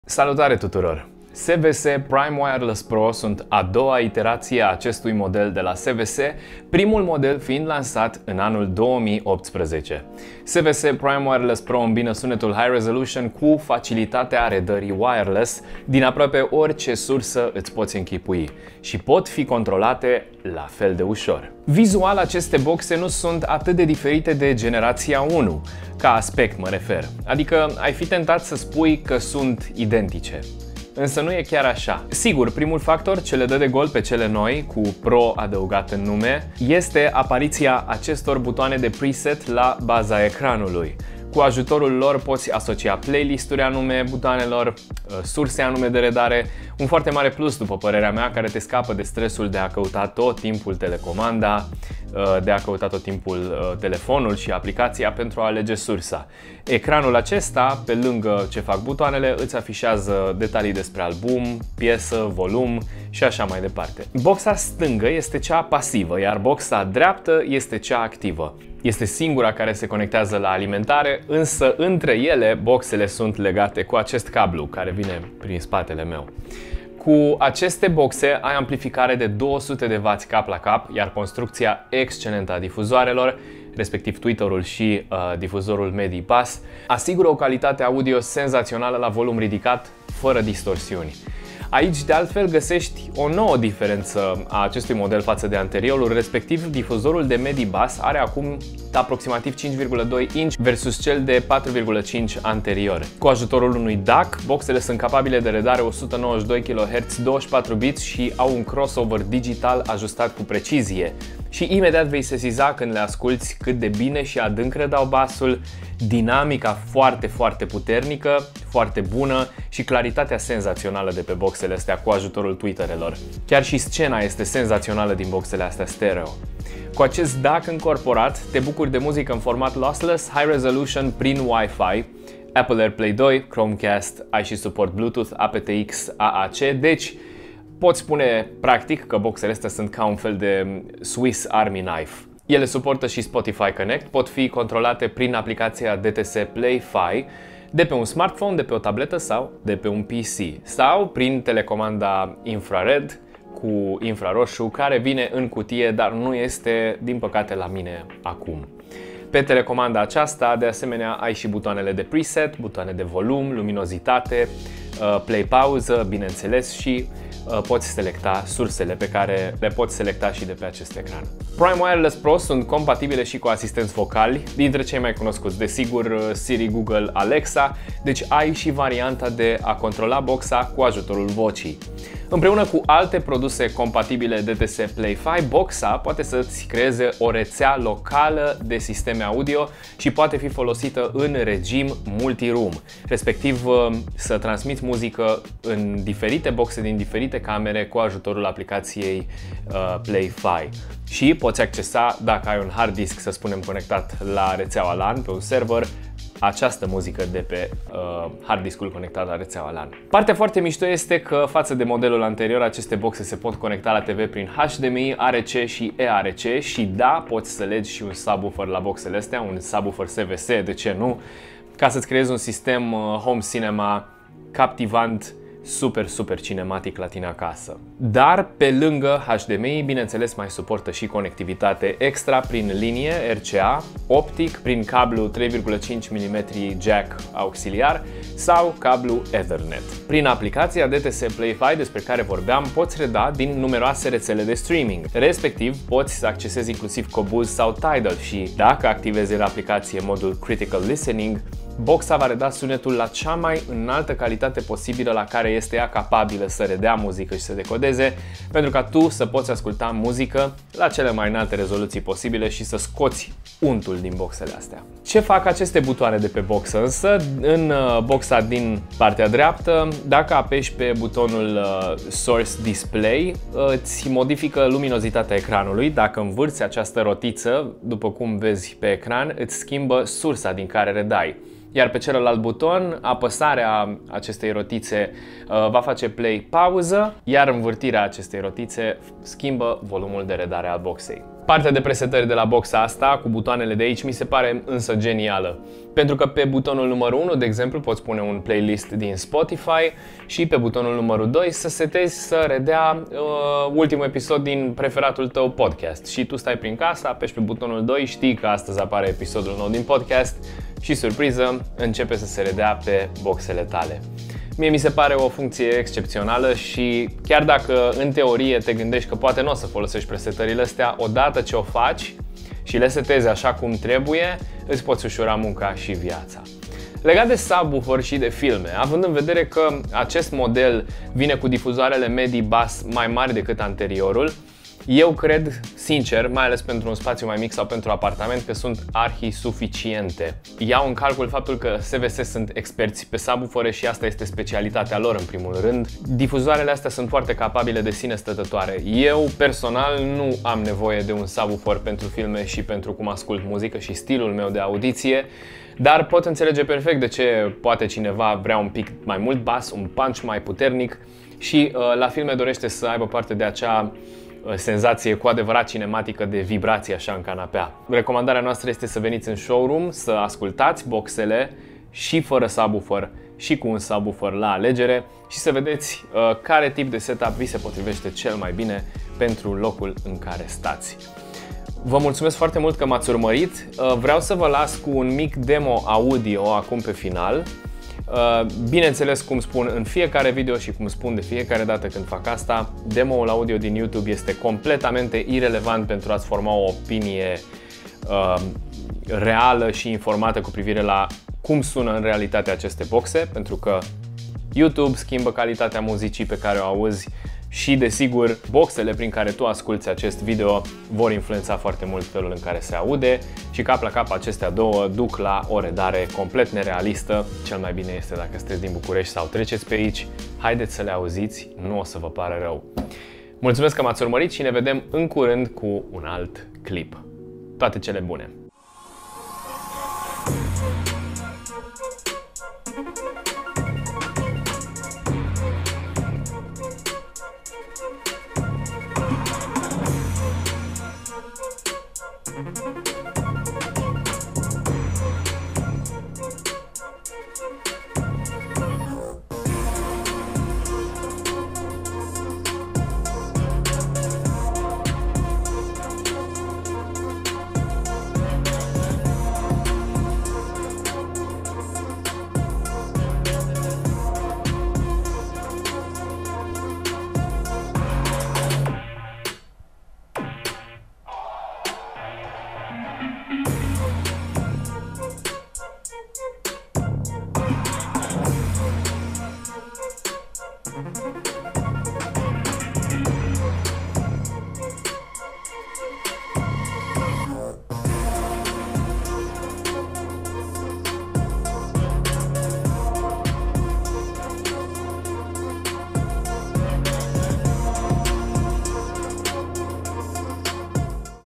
SVS Prime Wireless Pro - Ce nu fac boxele astea? All-in-one Stereo Hi-Fi system | AVstore
O pereche de boxe care ofera o acoperire de aproape suta la suta cu privire la sursele din care poti reada, sunet Hi=Res si functionalitati menite sa-ti usureze experienta audio.
Afla mai multe in clip si hai neaparat pe la noi sa le testezi!
Ai si un demo audio la capatul clipului. Asculta neaparat la casti.